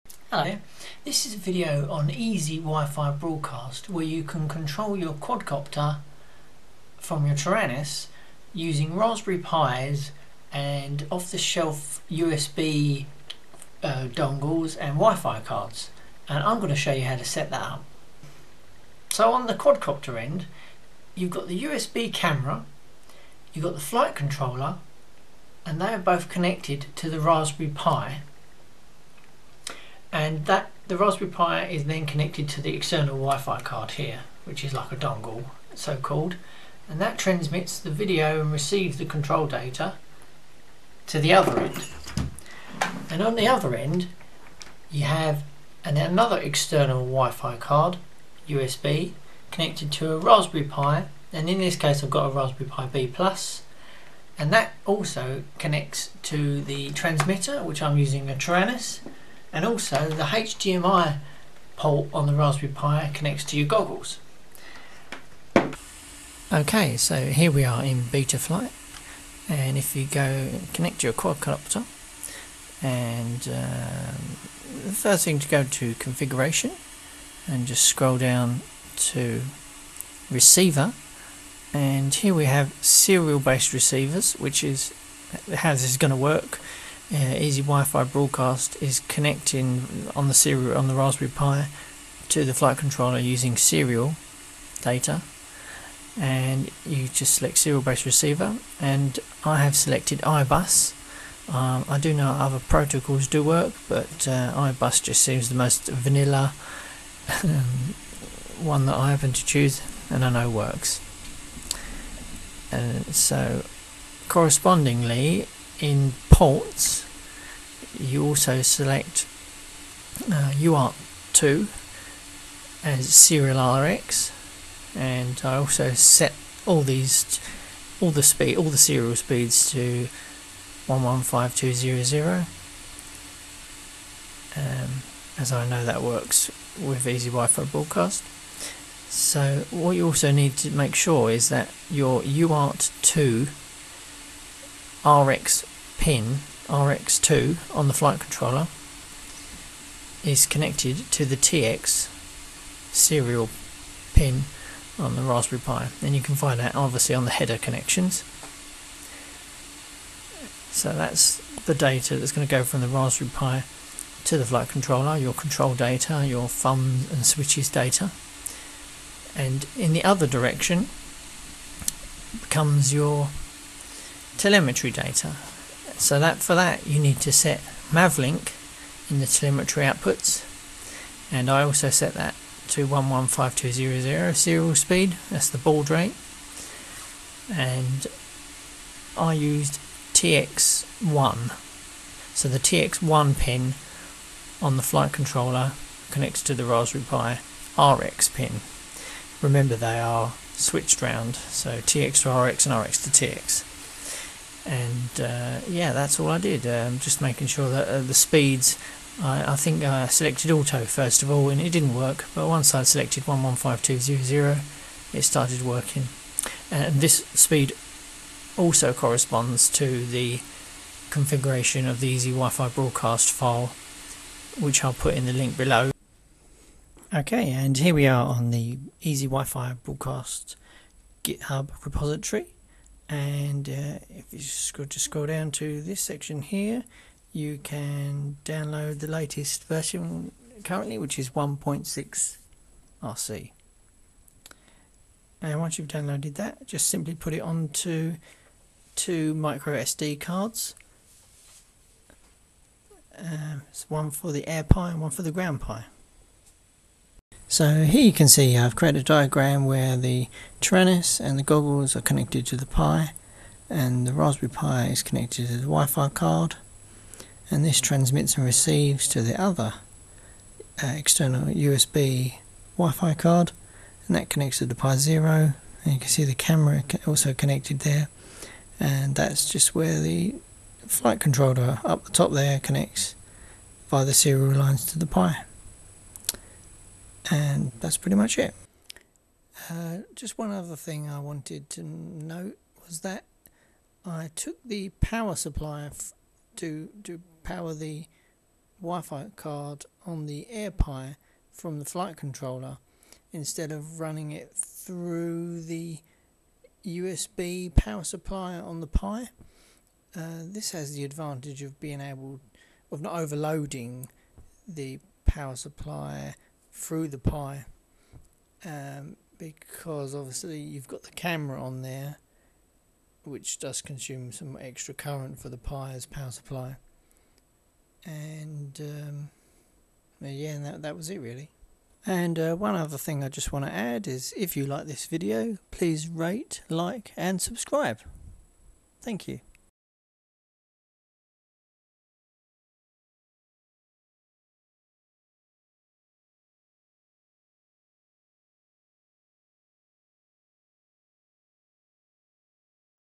How to setup EZ-WifiBroadcast with Betaflight
This is a tutorial on Ez-WifiBroadcast and how to connect it to Betaflight. You will need two Raspberry pies and two external network cards brands are specified on the Ez-WifiBroadcast git hub repository.